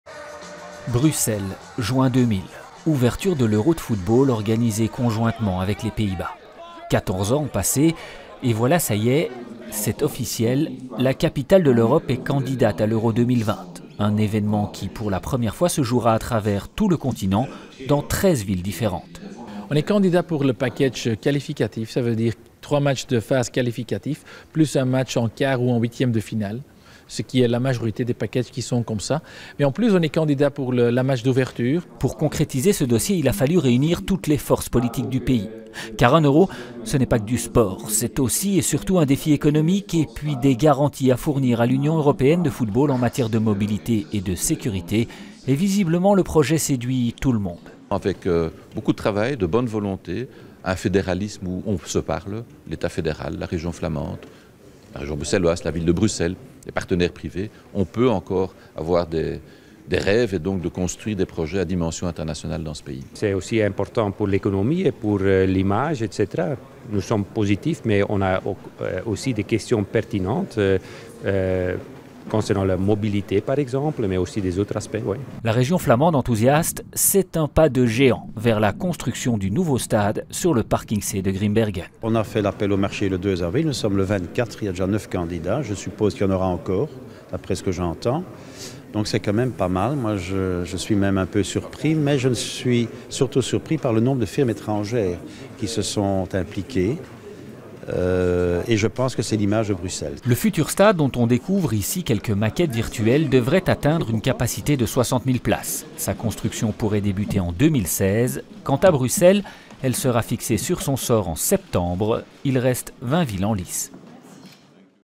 Bruxelles candidate pour l'Euro 2020
La Belgique et Bruxelles sont officiellement candidates pour l'accueil d'au moins trois matchs de l'Euro 2020 disputé dans 13 villes européennes.